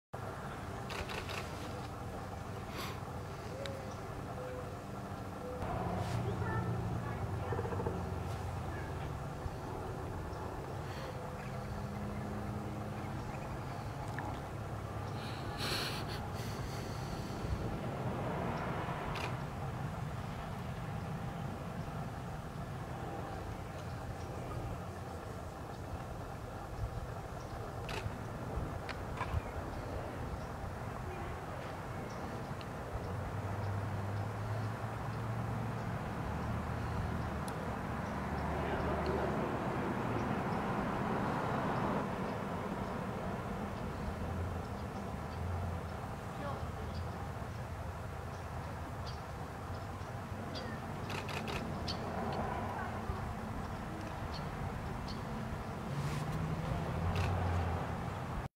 Great Horned Owl On The Nest
This Female Great Horned Owl has been sitting on the nest for about 30 days. The owlets should be here soon.